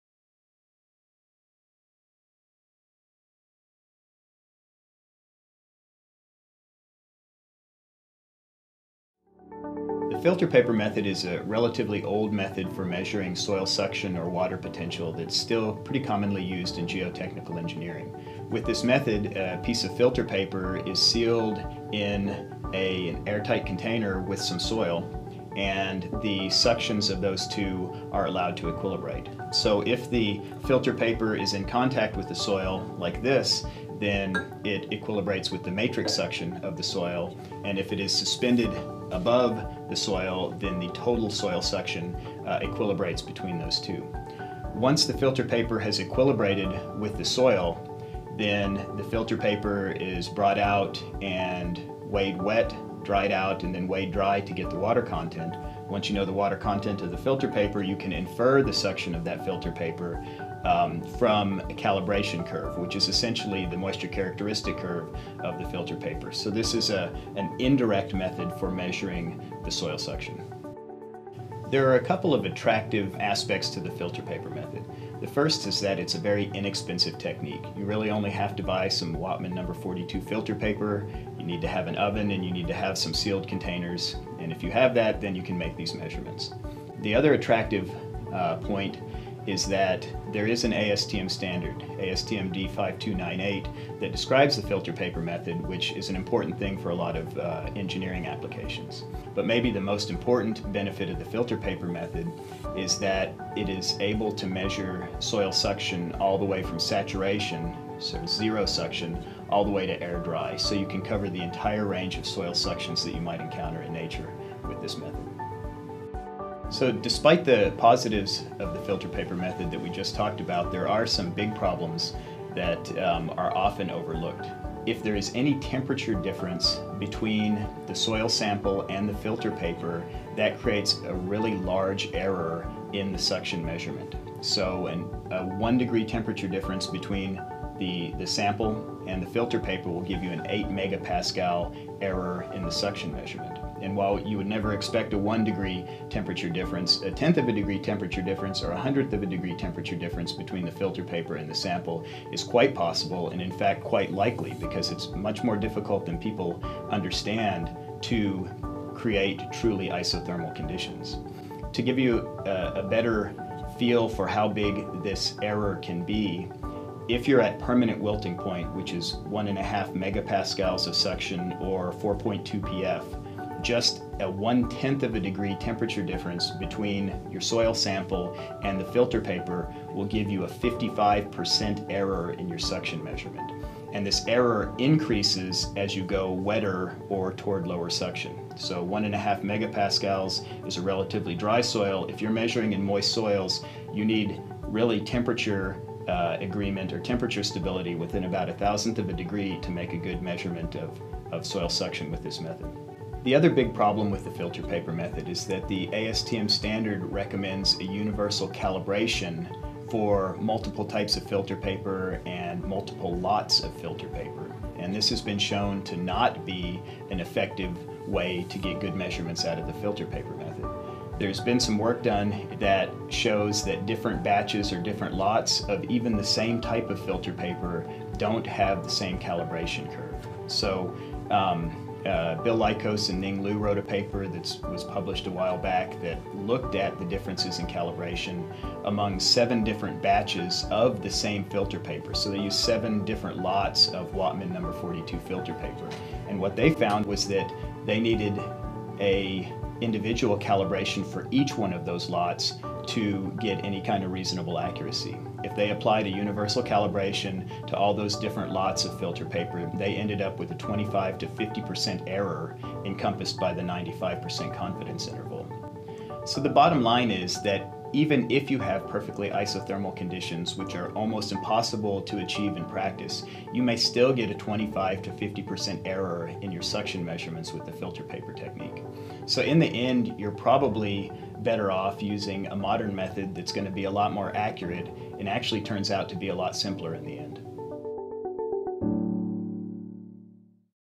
Filter Paper Method Pros & Cons for Measuring Soil Suction
The filter paper method is an older method for measuring soil suction or water potential that is still commonly used in geotechnical engineering today. With this method, a piece of paper is sealed in an airtight container with a soil sample and they are allowed to equilibrate.

In this video, Dr. Doug Cobos explains why the filter paper method can produce large errors in the suction measurement, and what you should use instead.